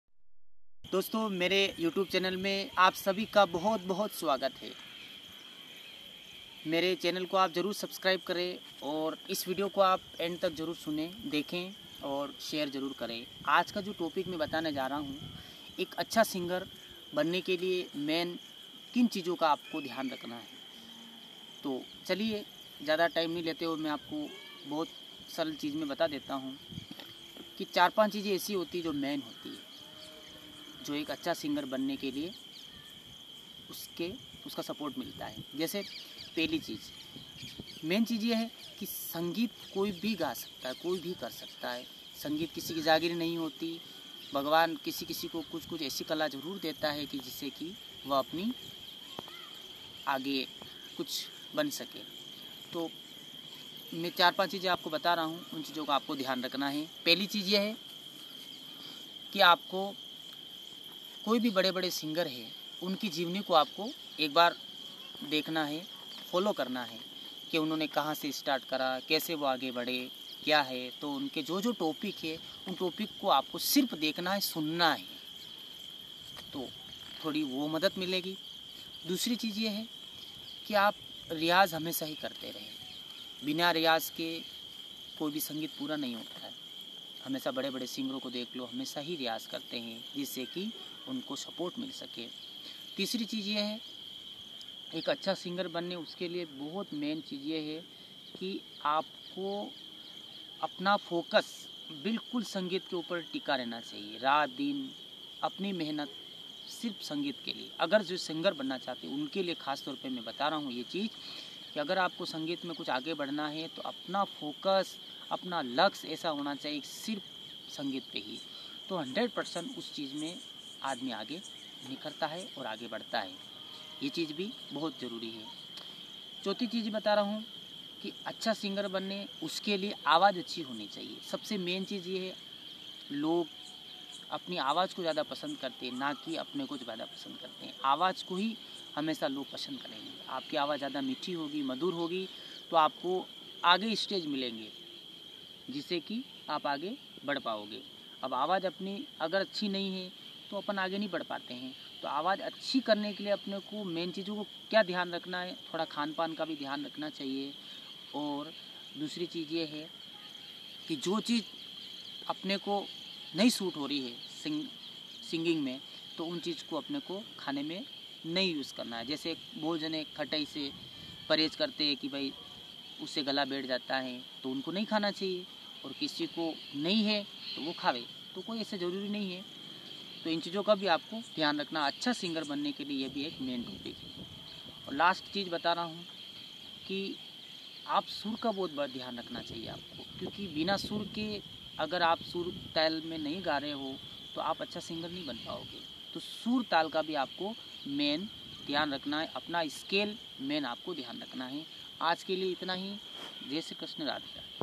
अच्छा सिंगर बनने के लिए क्या करना चाहिए What to do to be a Singer
Singer Motivation - सिंगर बनने के लिए क्या करे सिंगर बनने के लिए कुछ जरुरी बातें प्रिय दोस्तों एक अच्छा सिंगर वो ही बनसकता हे जो इन बातो को फॉलो करे 100% एक अच्छा संगीतकार बने Singer Motivation - Some important things to become a singer Dear friends, a good singer is the one who can follow these things 100% to be a good musician.